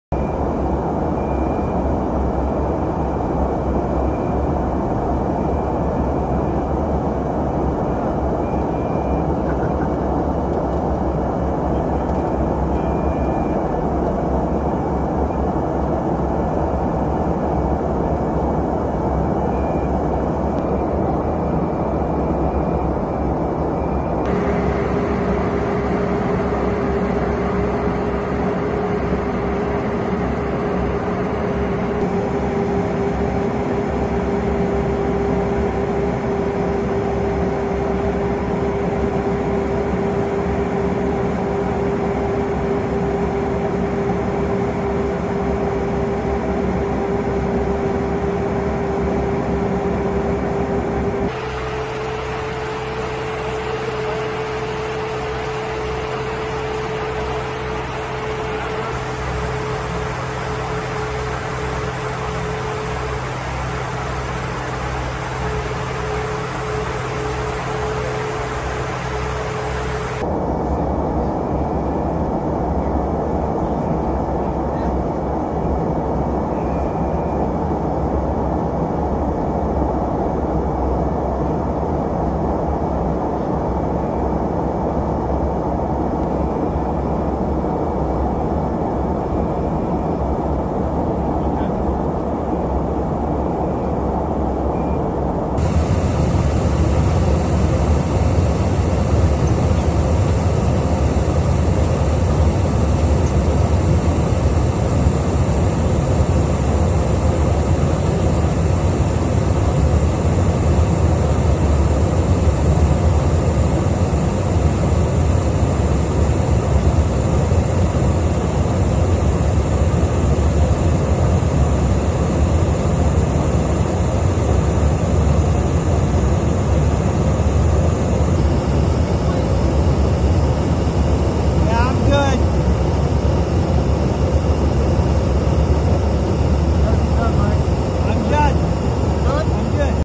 Американський літак вперше заправив винищувач російського виробництва
Американський літак-паливозаправник Boeing KC-135 Військово-повітряних сил Національної гвардії Вашингтона дозаправив три винищувачі Су-30МКМ Королівських ВПС Малайзії.